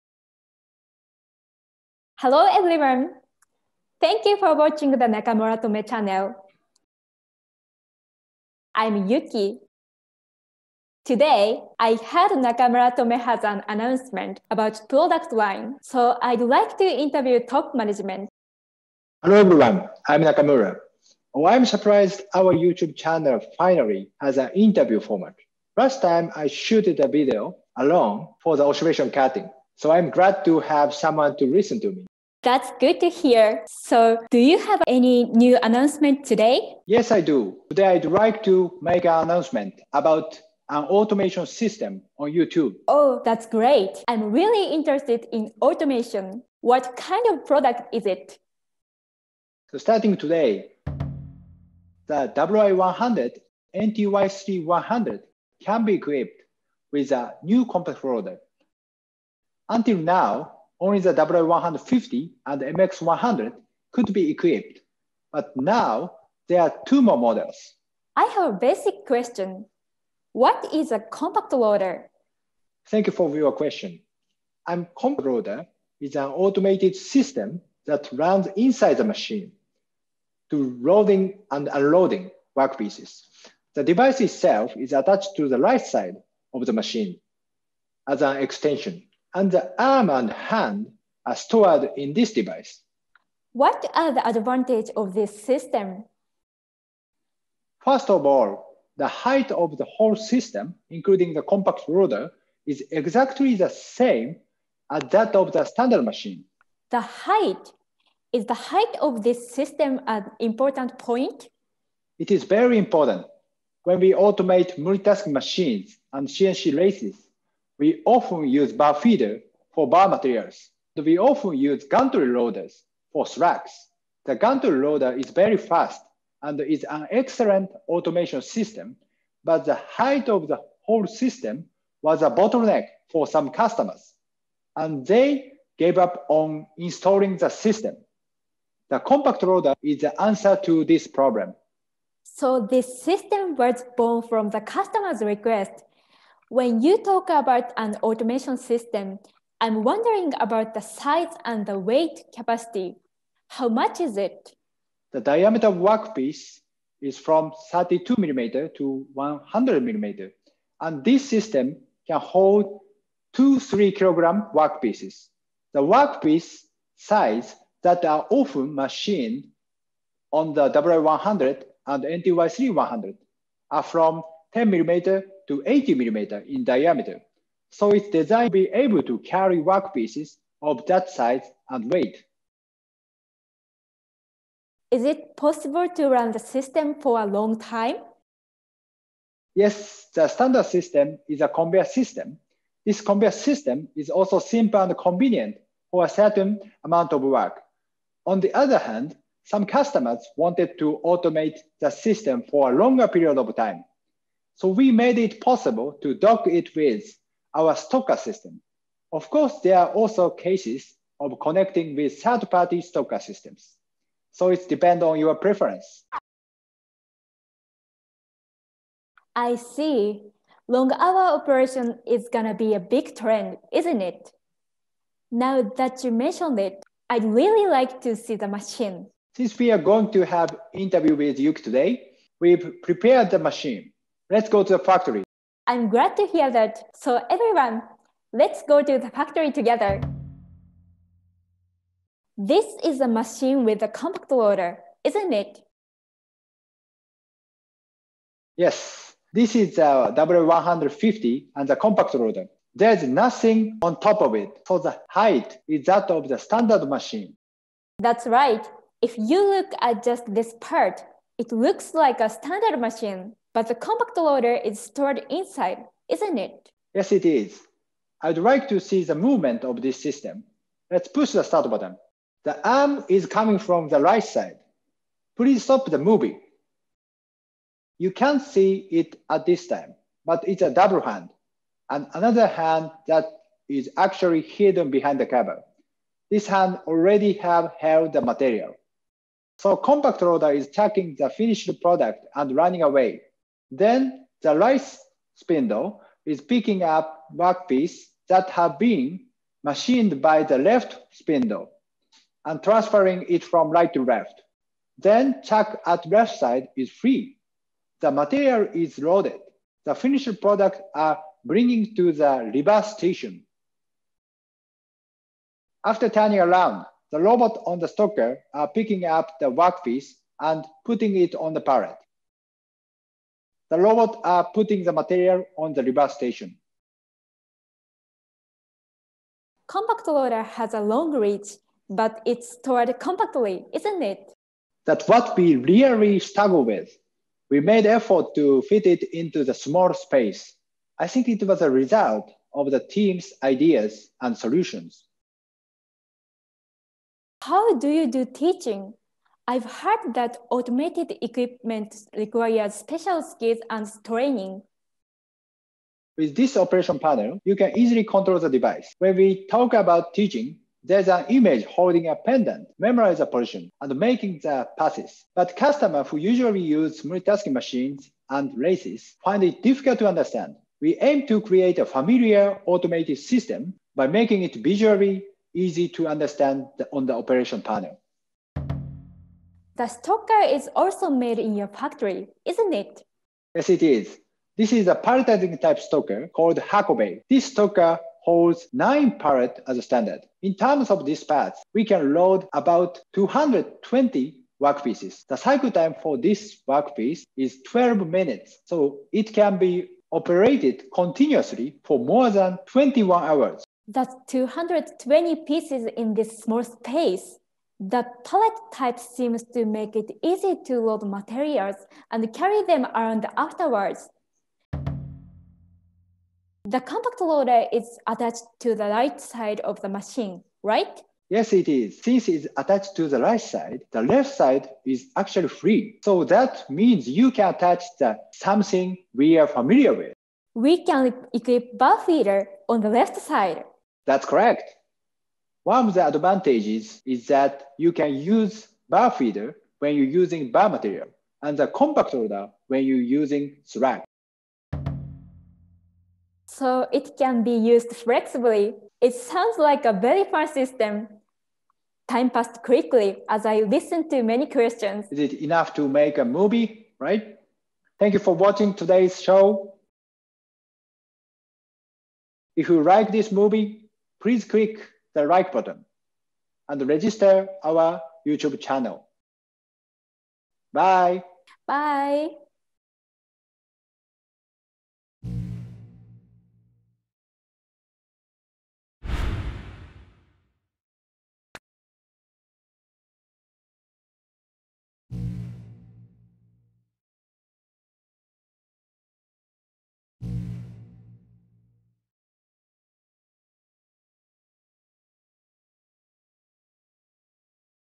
Product announcement | Expanding models equipped with the "Compact Loader"
The number of machines equipped with the compact loader are enhanced.
The height of a compactloader is the same as that of the standard machine,
In addition to the existing MX-100 and WY-150, the NTY³-100 and WY-100II will be equipped with this loader.
Two 3kg workpieces can be transported by double hand.
Either conveyor specification or stocker specification can be selected.
By combining with the multi-stack stocker Hakobei, automatic operation for a long time is possible.